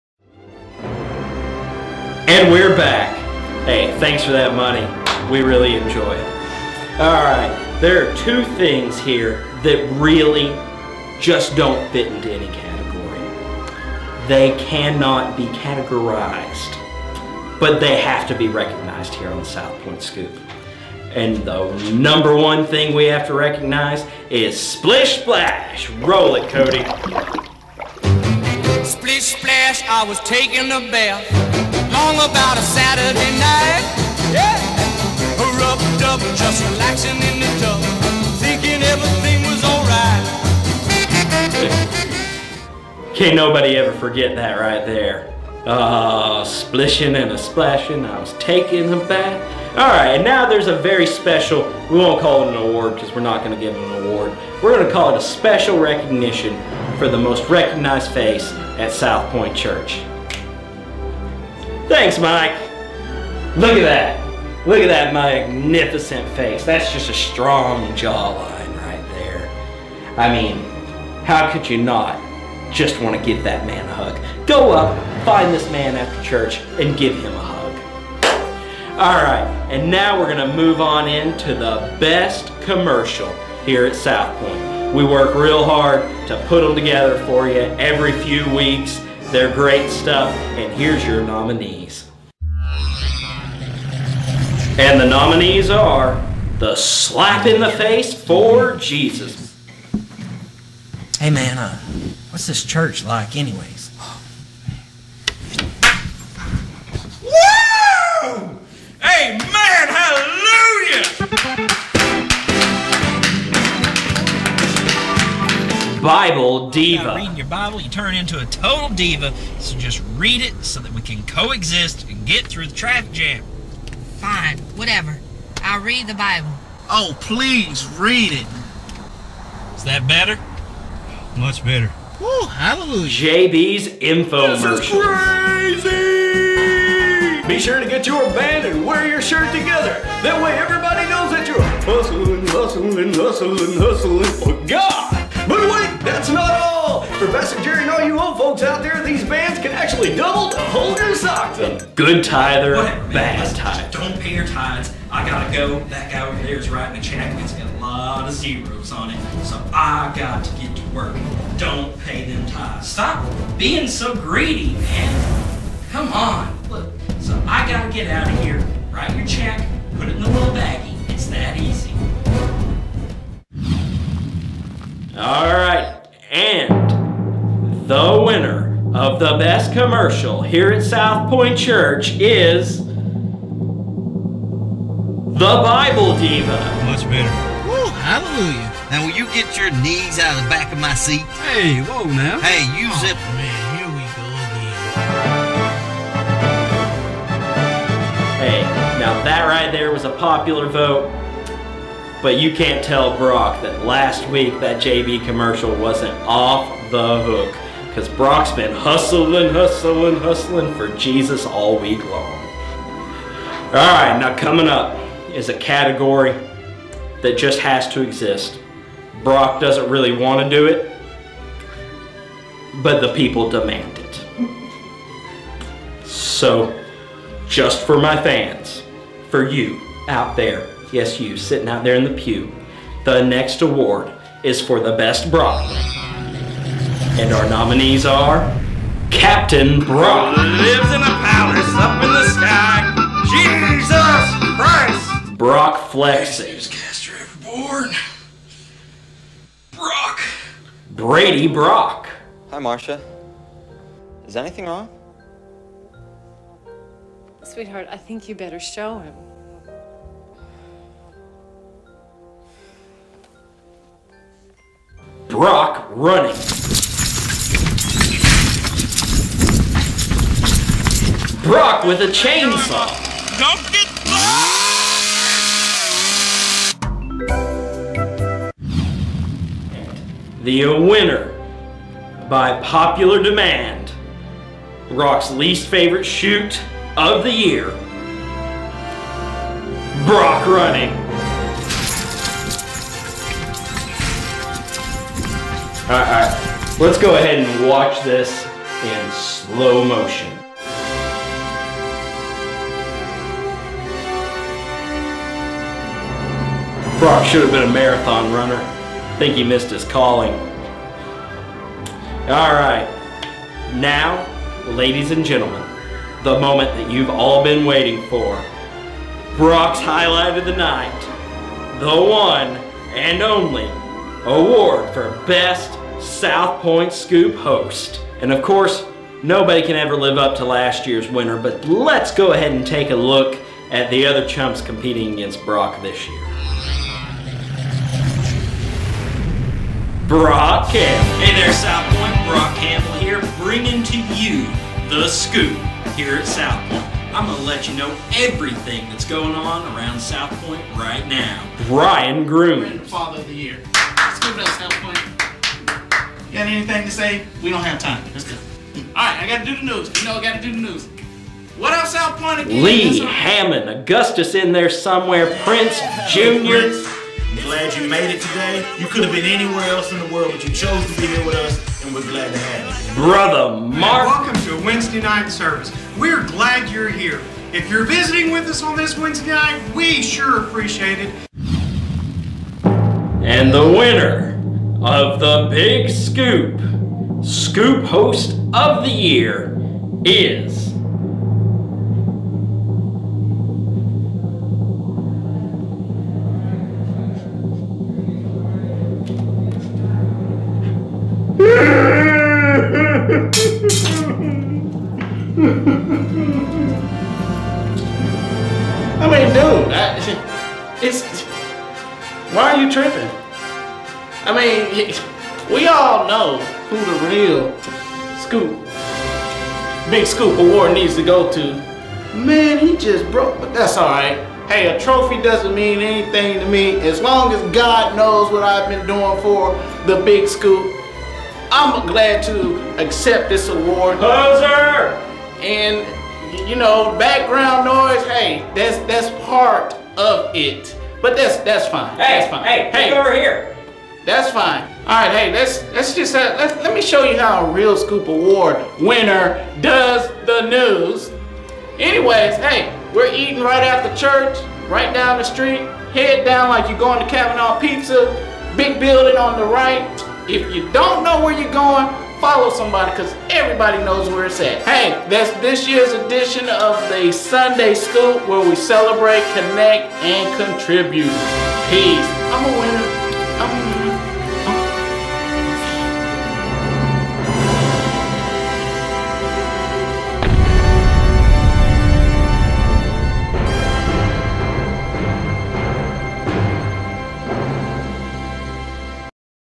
2011 Southpointe Scoop Awards Part 2
Here is part 2 of our 2nd Annual Southpointe Scoop Awards hosted by Brock Campbell.